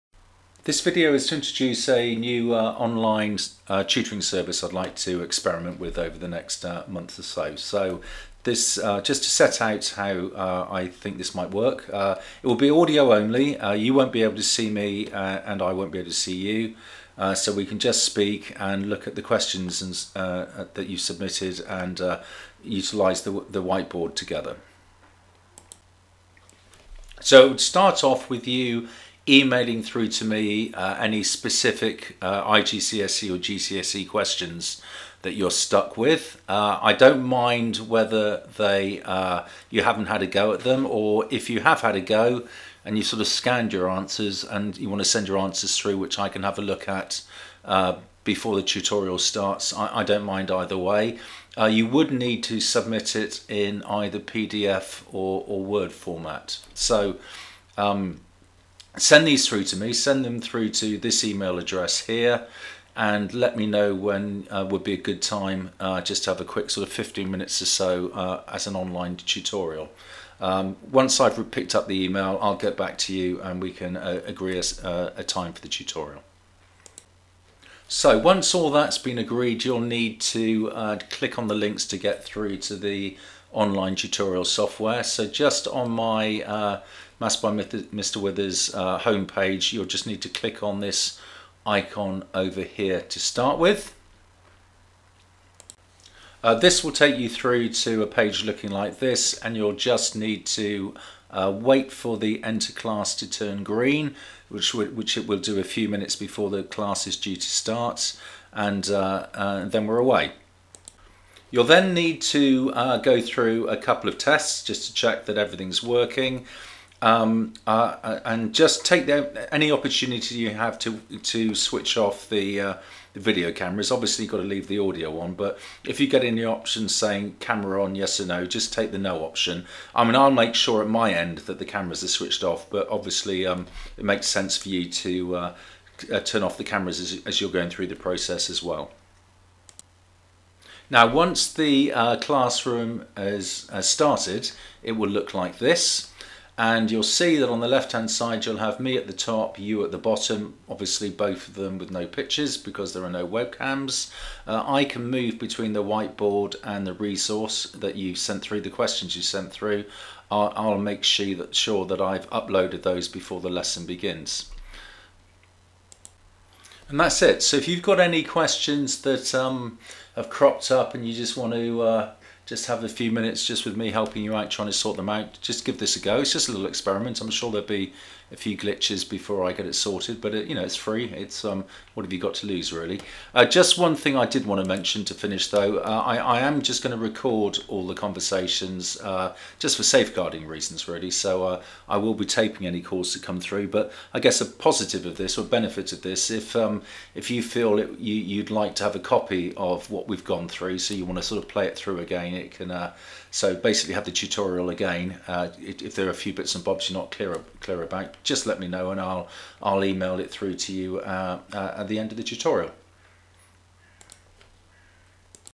Maths by Mr Withers Online Tutoring Trial
As we start a new academic year, I'm looking for a way to support my IGCSE/GCSE subscribers in addition to these videos. Quite often, just a few minutes of explanation, when stuck on a question, is all that is needed so I am exploring the option of offering free short one to one tutorials online. I am therefore trialing some new tutorial software. I am sure there will be a few teething problems to start with but it is free so why not give it a go!

These can either be blank or with your workings but they need to be in PDF or Word format. Please also suggest a suitable starting time for the brief tutorial. I will confirm the starting time then you can log on for the tutorial and we can hopefully work through your queries!

 Please note that for safeguarding reasons these tutorials will be audio only (with all webcams switched off) and I will also be recording all tutorials.